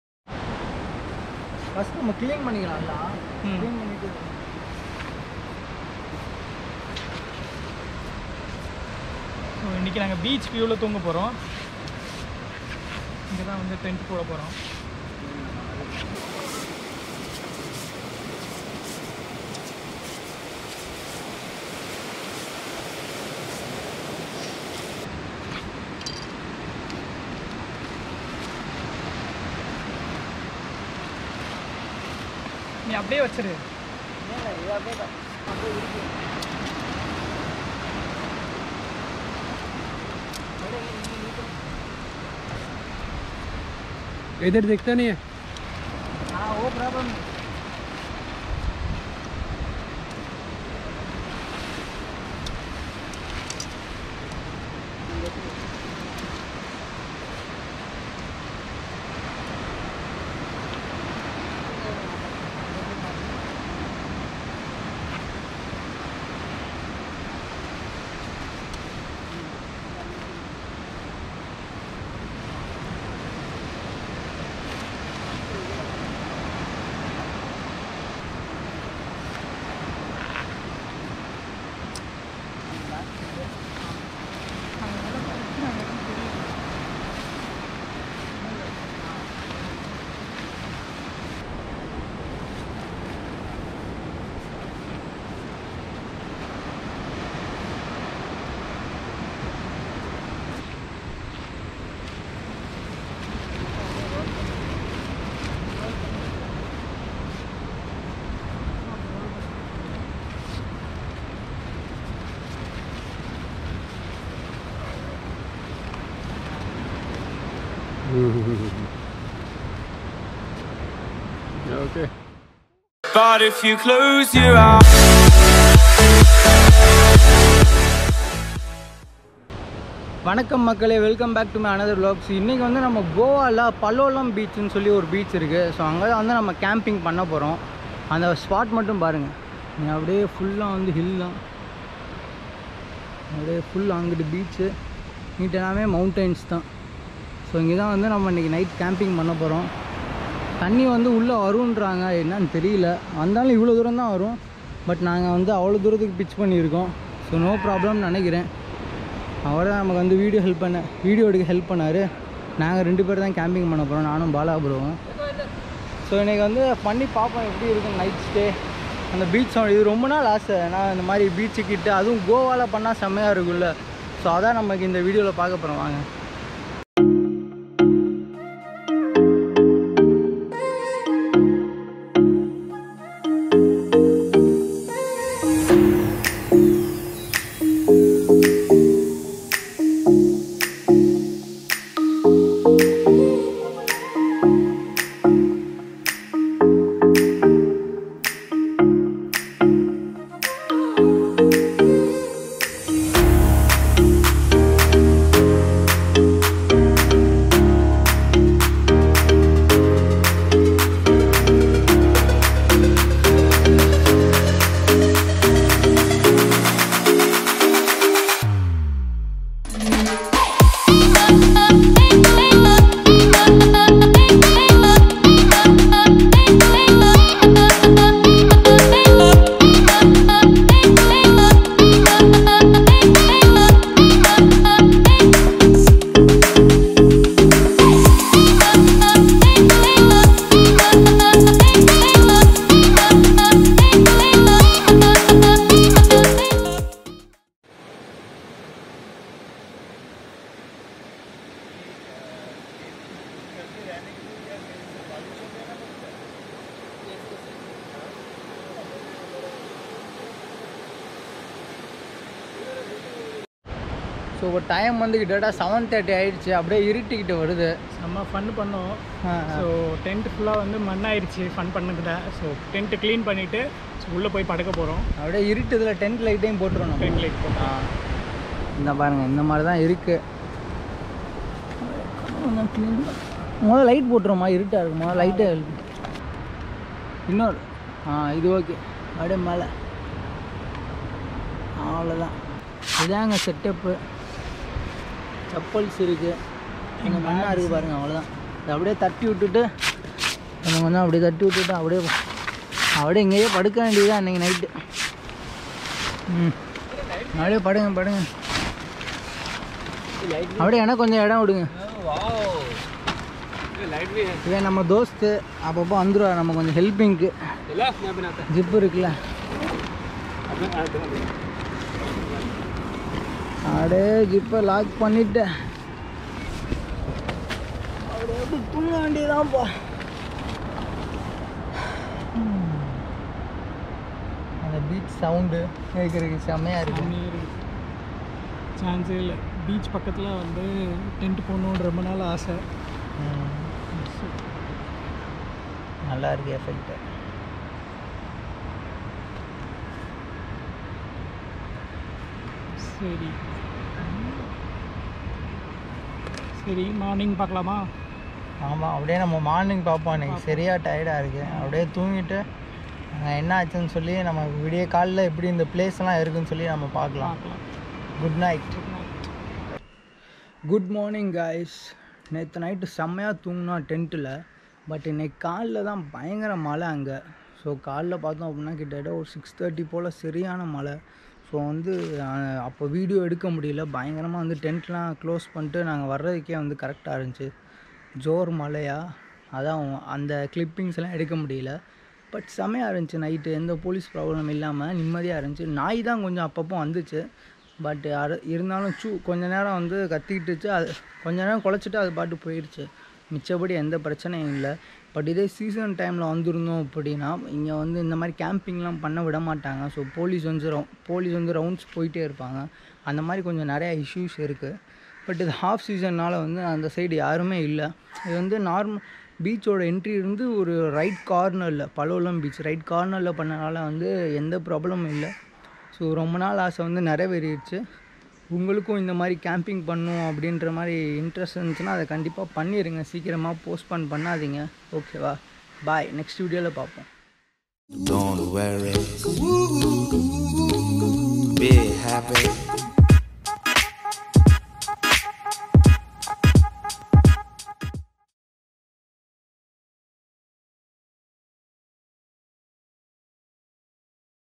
Tentstay in Beach Goa Palolem Beach | Camping in South Goa
Camping in Beach is always in the Bucket list of most of the travellers, Finally it came true to me in this beautiful Palolem Beach in South Goa.

Camping in Goa!
Goa places to visit!